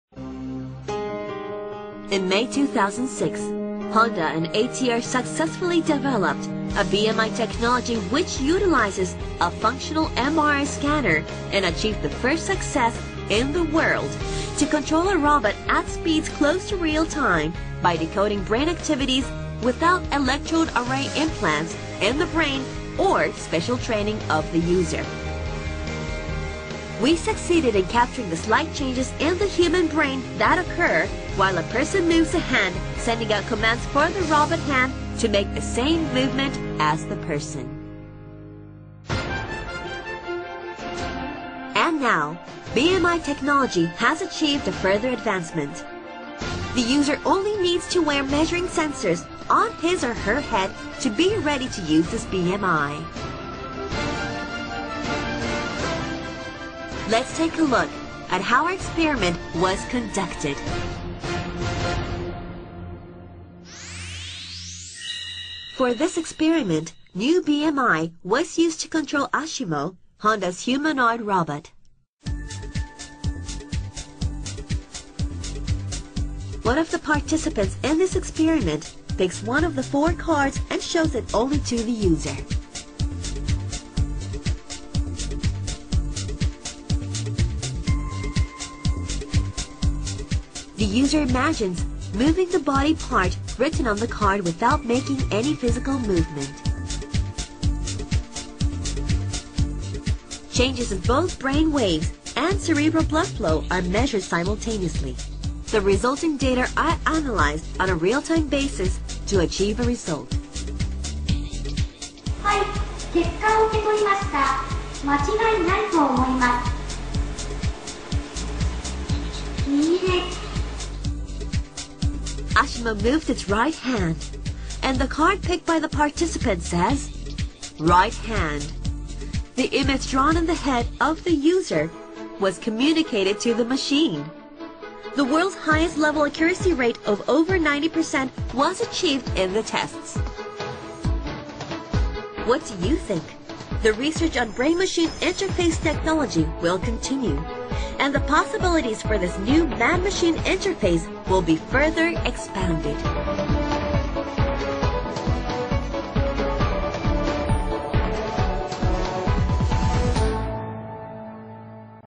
Honda develops brain interface (BMI) for robot control
The research wing of Honda Motor has co-developed a brain machine interface (BMI) system that allows a person to control a robot through thought alone.